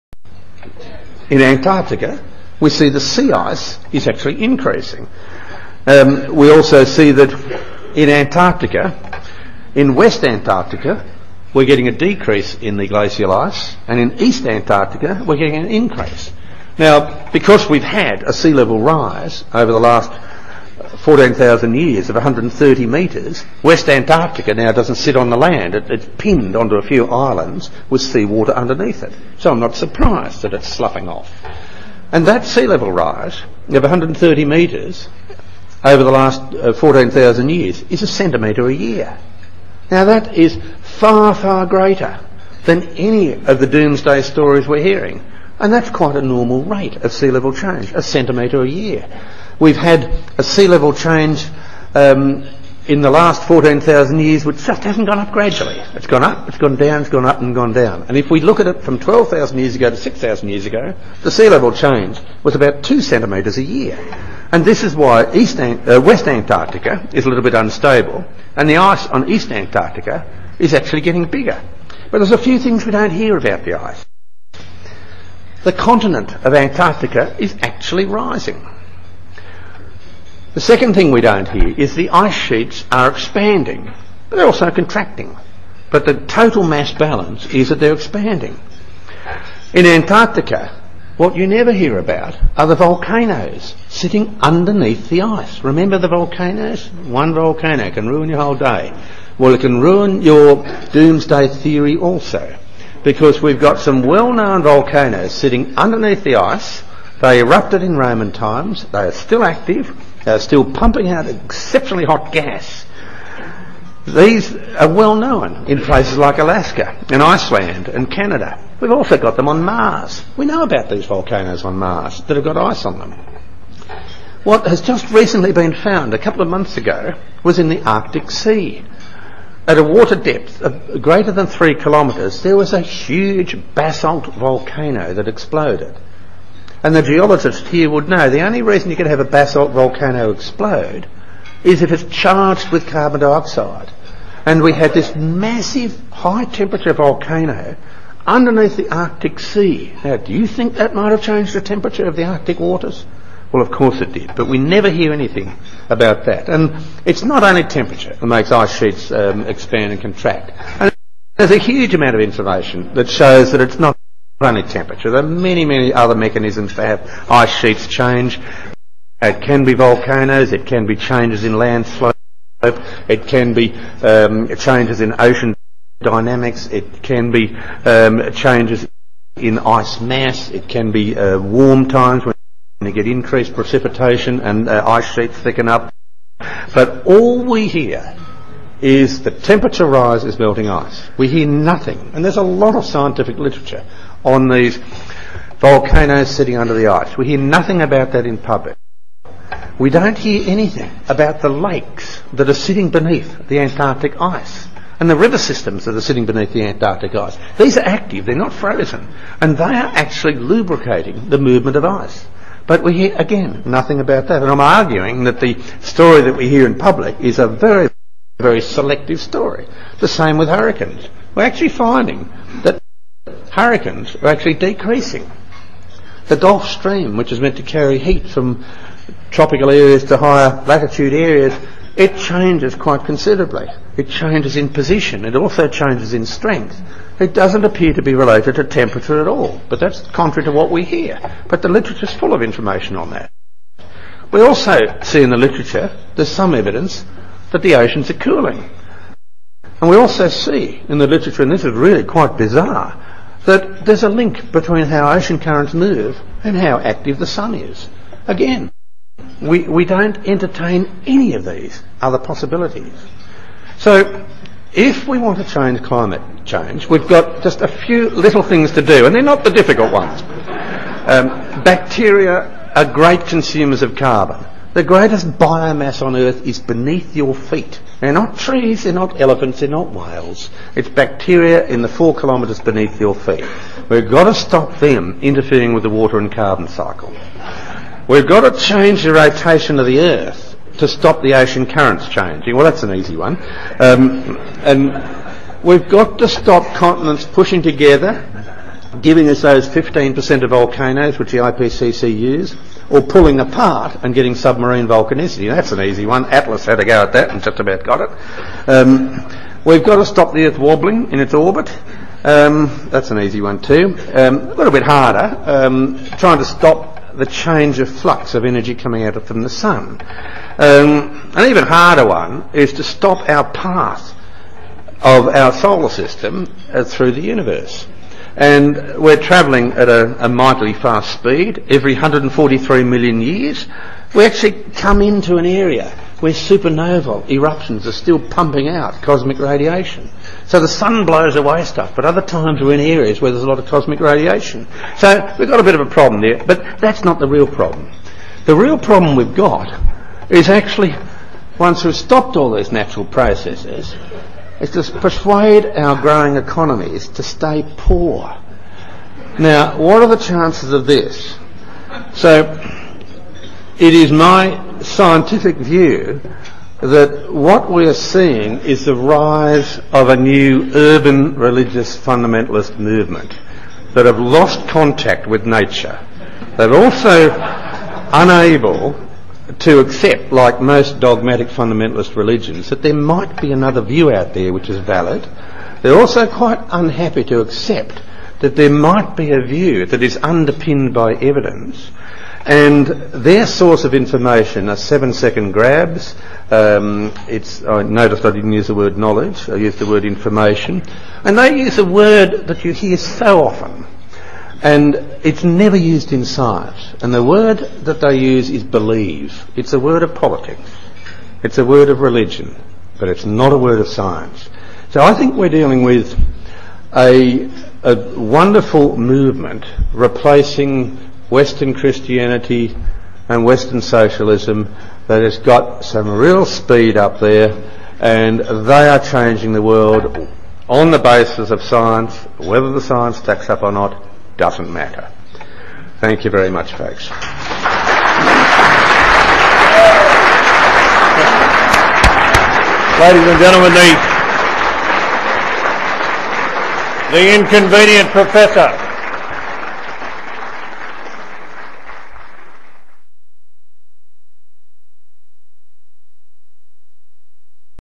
Human Induced Climate Change - Ian Plimer (part 5 of 5)
Plimer is currently Professor of Mining Geology at the University of Adelaide. He was previously a Professor in the School of Earth Sciences at the University of Melbourne. He is also a prominent member of the Australian Skeptics. He was awarded the Clarke Medal by the Royal Society of New South Wales in 2004.